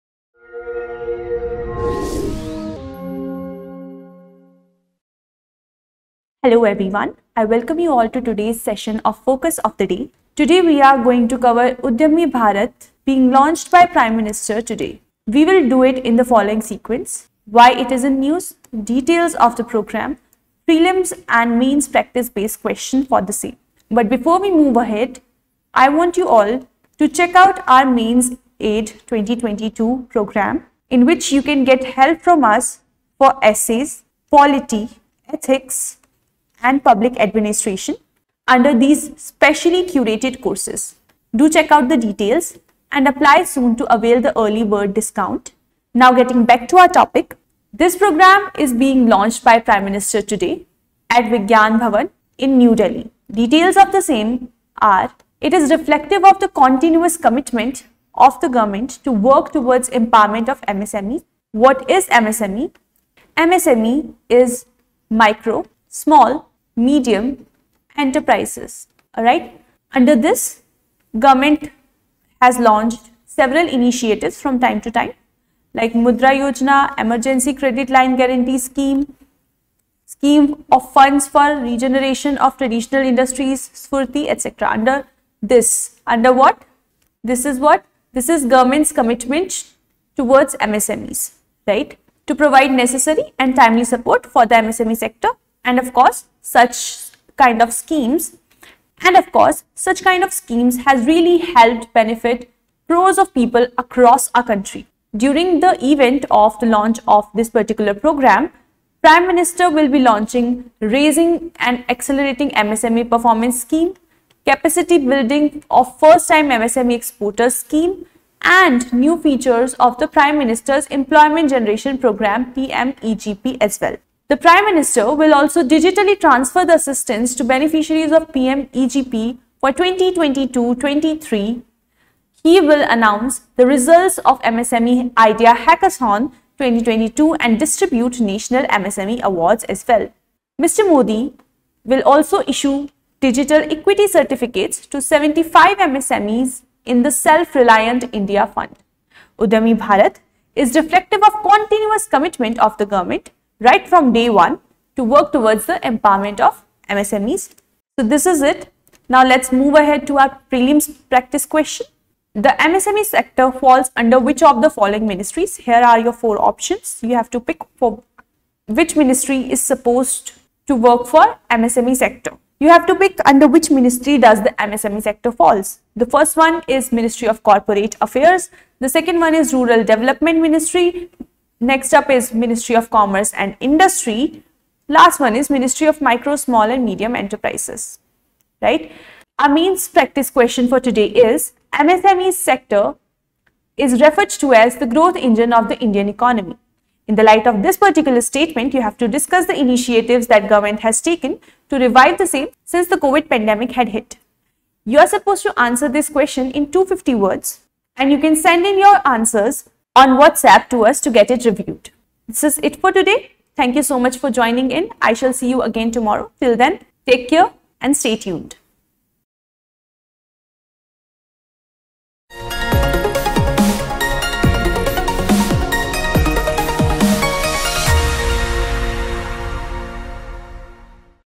UDYAMI BHARAT | FOCUS OF THE DAY | UPSC CSE | Analyst IAS
In today's focus of the day we have covered about UDYAMI BHARAT

01, R Block, New Rajinder Nagar, Delhi -60
land mark - DI khan School Campus, R block, Delhi 60